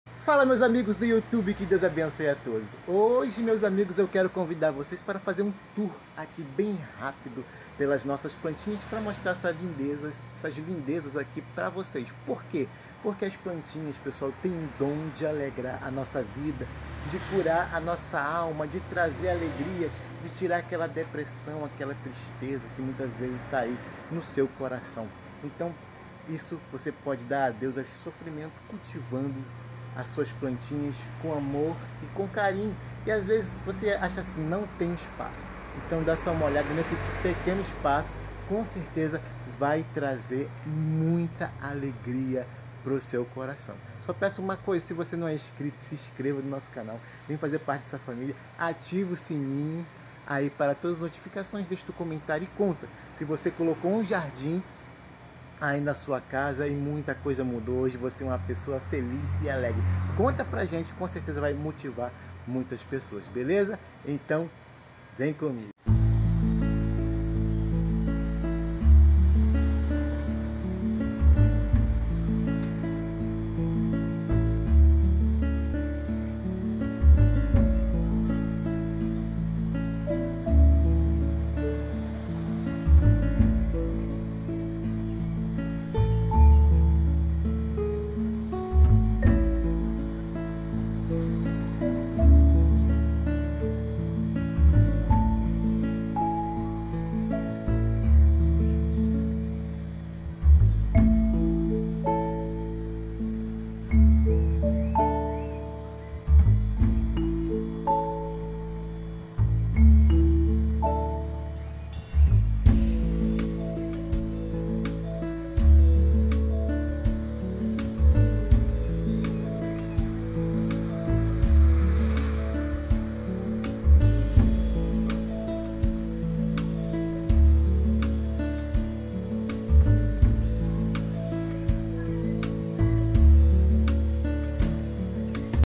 Tour Pelas minhas plantas Esse Lugar Cura até Depressão
as plantinhas são Criação de Deus pra alegrar nosso coração por isso essas cores fazem tão bem a gente